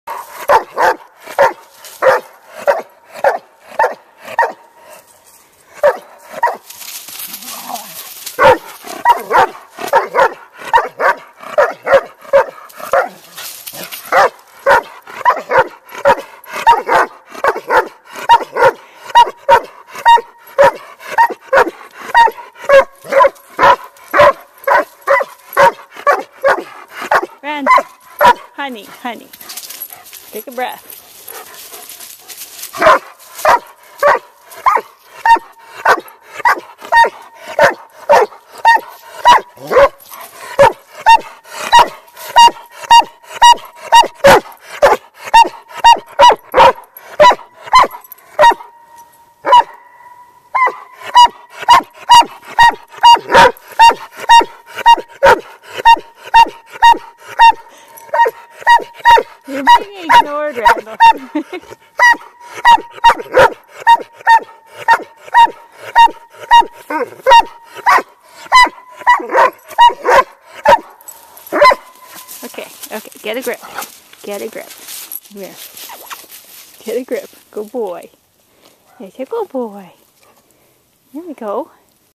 Close Talker
The riffraff think Rambo's a bit of a doofus for yelling in their ears, and choose to ignore him. (This is a ridiculous bark for a hundred pound doberman, too!)